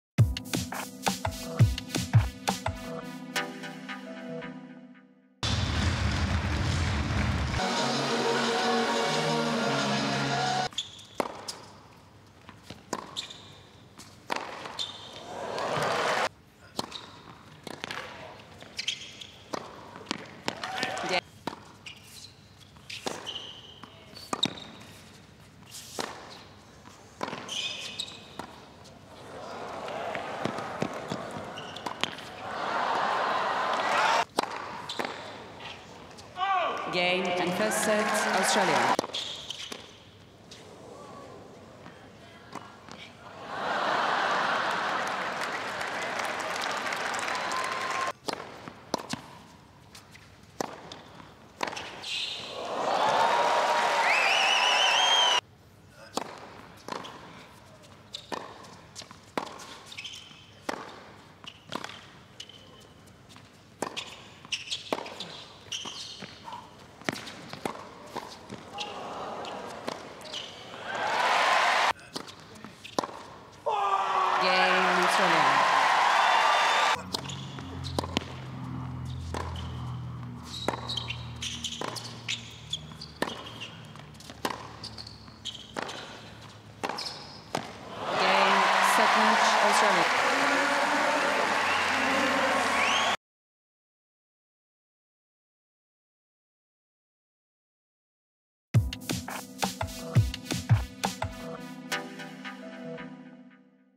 Nick Kyrgios v Adam Pavlasek highlights (RR) - Mastercard Hopman Cup
Nick Kyrgios has put Australia in the box seat with a 7-5 6-4 win over Czech Republic's Adam Pavlasek.